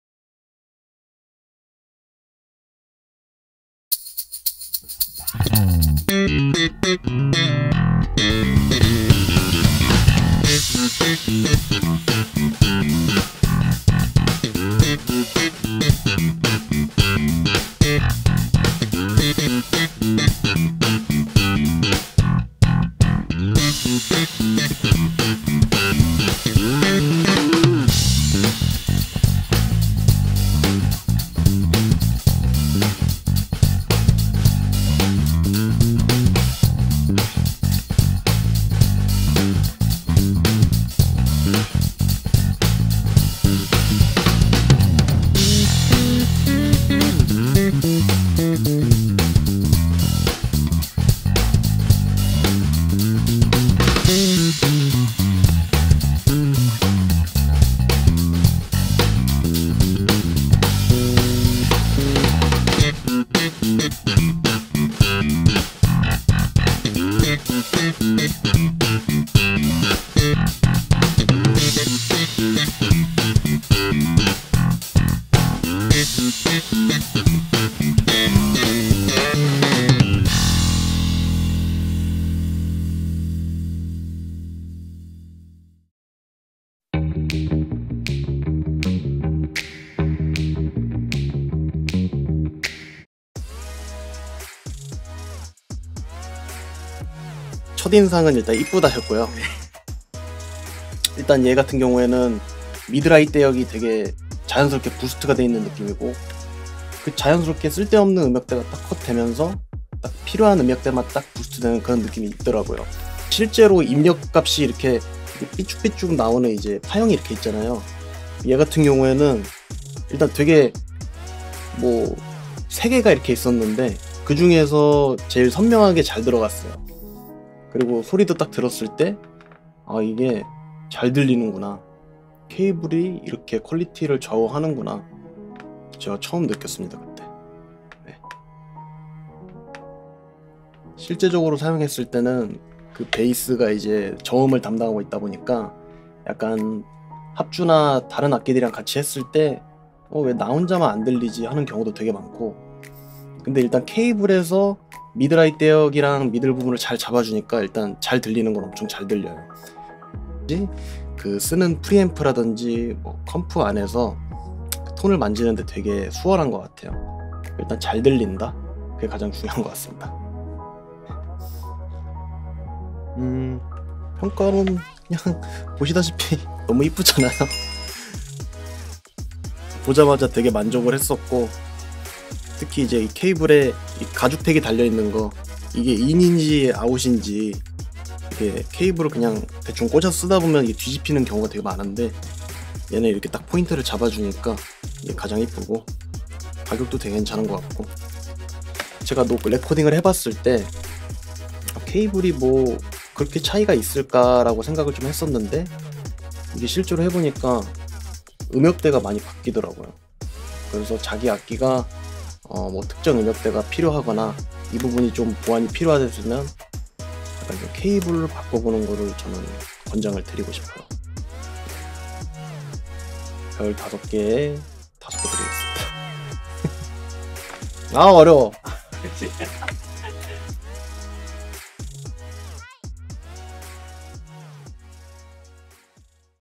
1963 Fender Jazz Bass X Sound Lap Studio Cable
전천후 스튜디오 케이블 홍보영상 입니다!
미들부터 미들하이대역이 중심으로 잡혀있는 스튜디오 케이블 입니다.
구매문의 instar . @soundlab_jeoncheonhoo